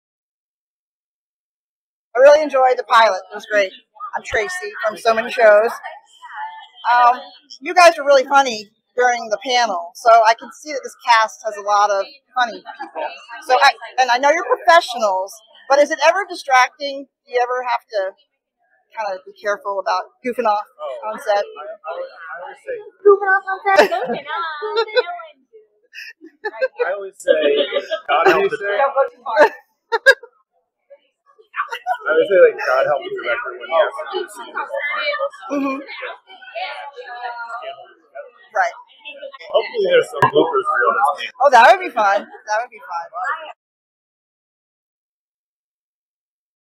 True Lies actors Erica Hernandez and Mike O'Gorman on working with a really funny cast
True Lies actors Erica Hernandez (Maria) and Mike O'Gorman (Luther) talked to us on the red carpet at SCAD TVfest about having fun with their castmates on set. The cast is chock full of actors who are naturally very funny. What happens when they all get together to film a scene? We really would like to see a blooper reel!
Actors Erica Hernandez, Mike O'Gorman, Ginger Gonzaga and Steve Howey attend the 11th annual SCAD TVfest in Atlanta, GA, Feb. 9-11, 2023. The Savannah College of Art and Design’s annual signature event celebrates all things television and streaming.